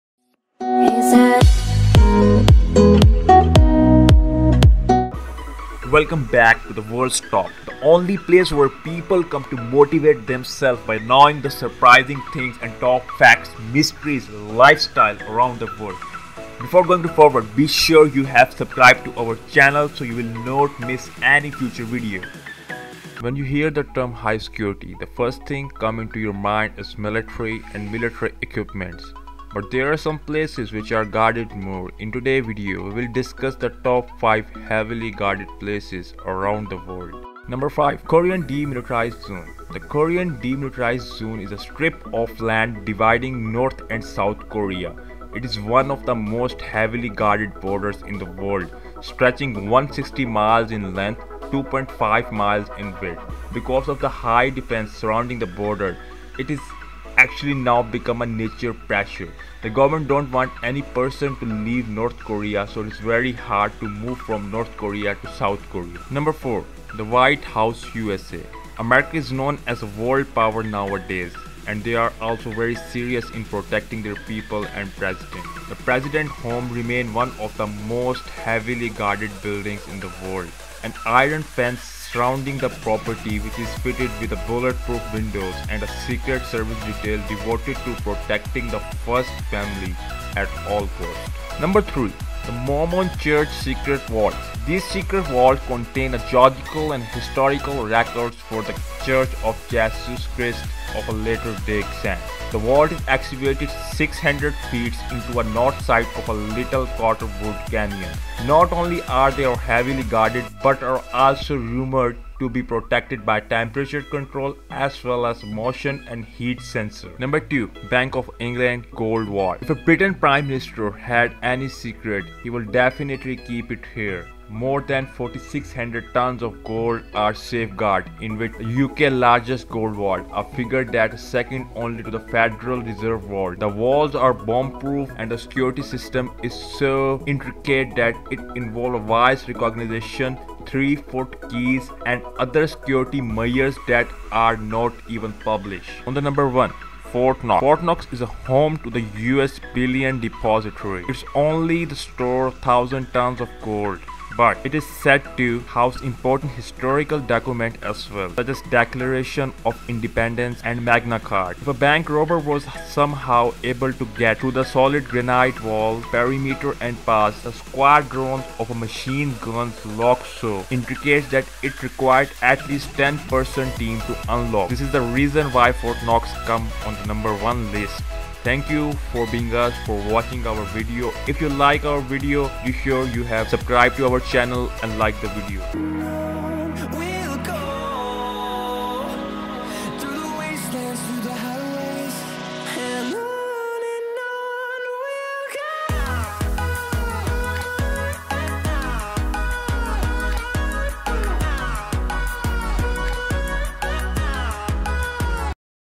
top 5 places which are heavily guarded around the world
intagram :zunair_z

you should look out at those videos
5
4
3
2
1

wen you hear the term high security the first thing come into your mind is military and military equipments but thr are some places which are guarded more
in today video we will discuss the top 5 heavily guarded places on the earth

 no 5
 Korean Demilitarized Zone
The Korean Demilitarized Zone is a strip of land dividing North and South Korea. It is one of the most heavily guarded borders in the world, stretching 160 miles in length and 2.5 miles in width. Because of the high defense surrounding the border, it has actually now become a nature pressure.
the goverment dont want any person to leave north korea so its very hard to move from north korea to south korea

4. The White House, USA american is know as world power now a days and they are also very serious in protecting
thier people and president

The President's home remains one of the most heavily guarded buildings in the world. An iron fence surrounds the property which is fitted with bullet proof windows and a secret service detail devoted to protecting the First Family at all costs.

3- The Mormon Church’s Secret Vaults
These secret vaults contain genealogical and historical records for The Church of Jesus Christ of Latter-day Saints. The vault is excavated 600 feet into the north side of Little Cottonwood Canyon,  Not only are they heavily guarded, but are also rumored to be protected by temperature control, as well as motion and heat sensors.

2- Bank of England gold vault
If Britain’s Prime Minister had a secret, he would want to keep it here. More than 4,600 tons of gold are safeguarded in what is the UK’s largest gold vault. A figure that’s second only to the Federal Reserve Vault. The walls are bombproof and the security system is so intricate that it involves voice recognition, 3 foot keys and other security measures that aren’t even published.

1-Fort Knox
Fort Knox is home to the US Bullion Depository. It not only stores thousands of tons of gold, but, it is said to house important historical documents as well, such as the Declaration of Independence and the Magna Carte. If a bank robber was somehow able to get through the solid granite wall perimeter and past the squadrons of machine-gun wielding guards and armed military, they would still have to contend with a 22-ton vault blast door held shut by a lock so intricate that it requires a 10 person team to unlock.